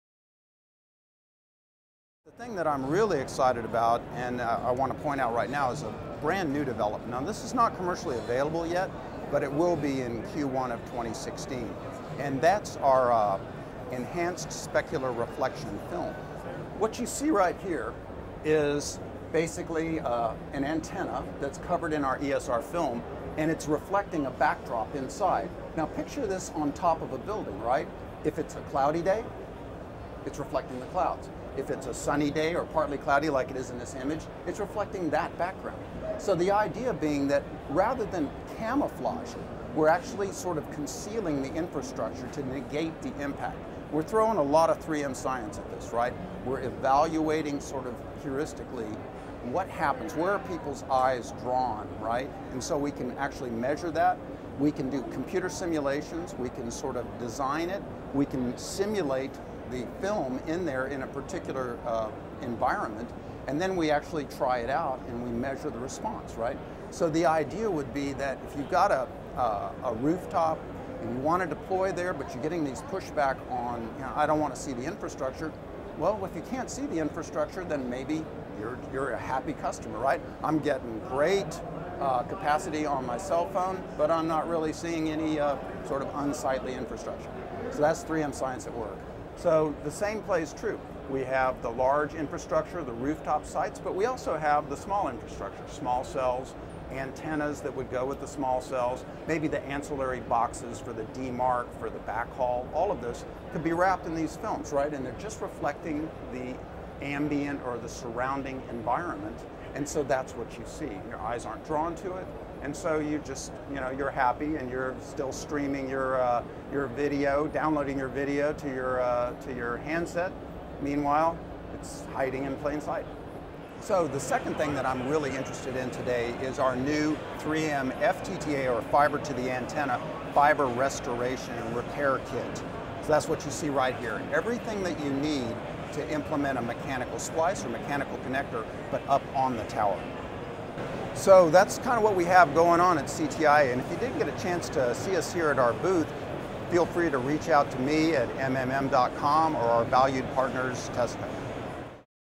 CTIA - TESSCO Partner 3M
3M discusses innovative solutions for Wireless at CTIA 2015